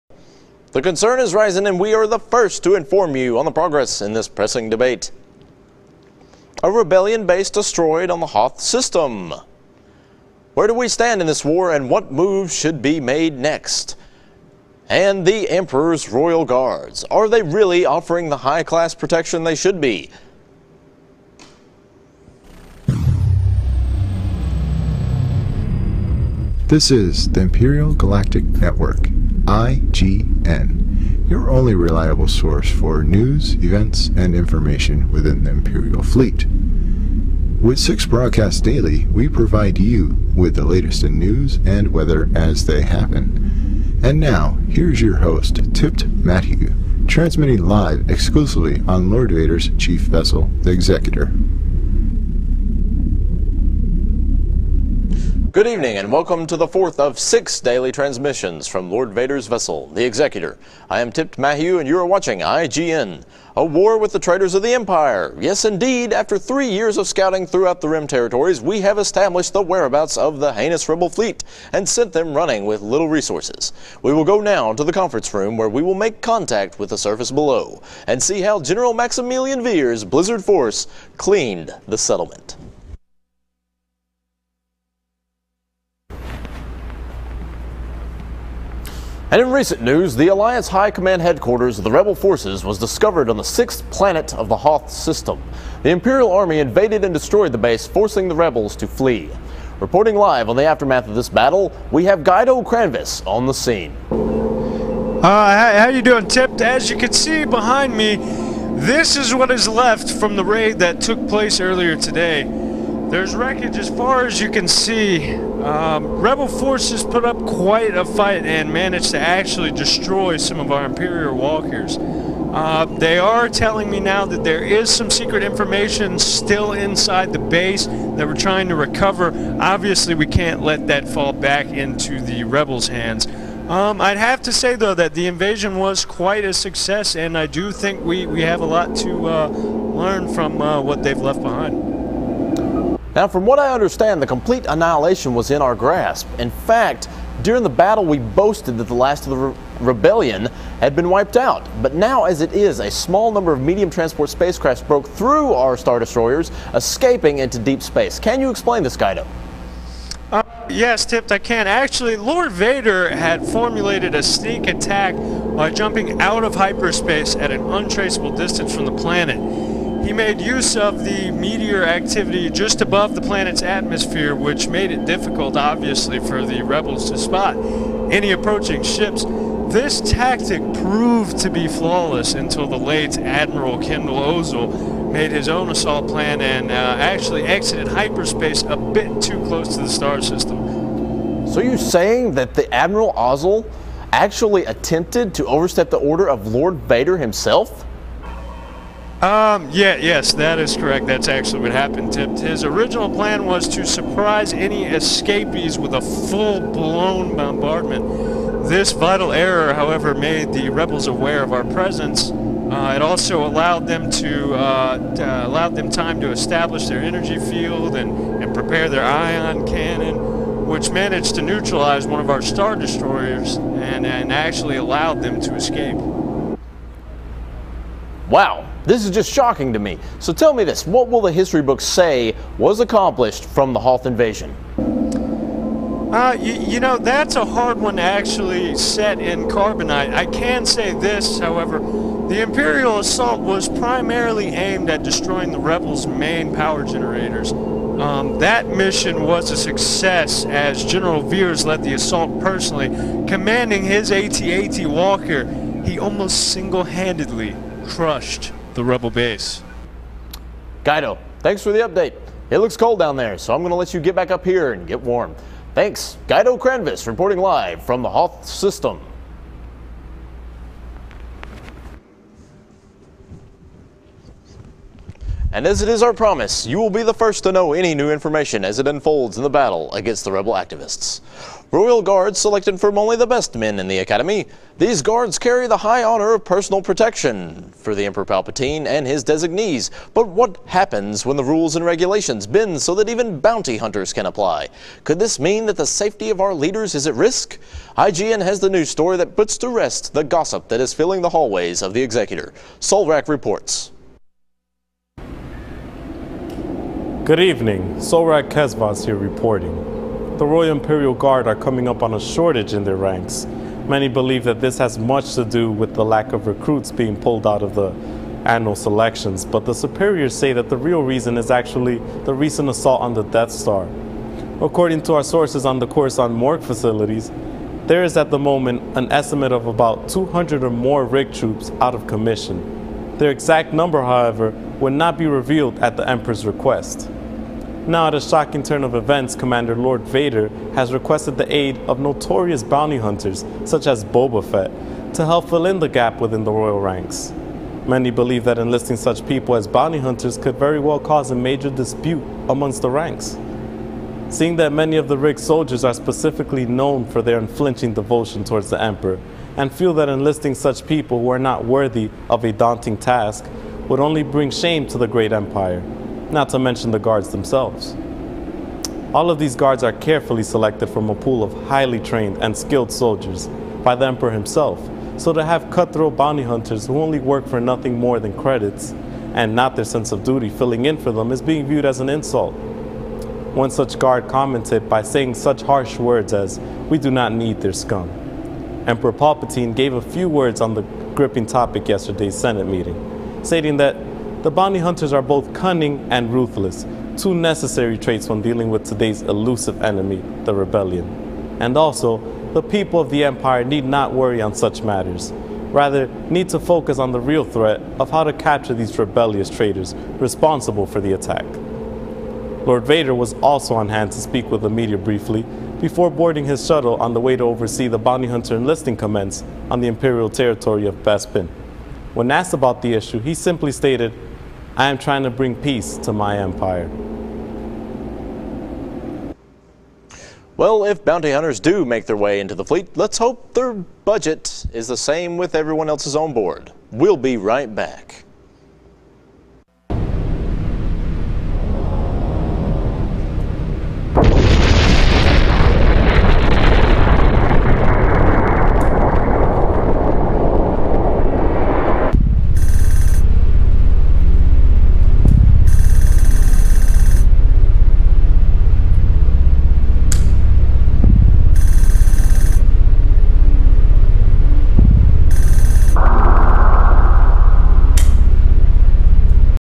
Star Wars News Part 1
Star Wars News Show for a college final project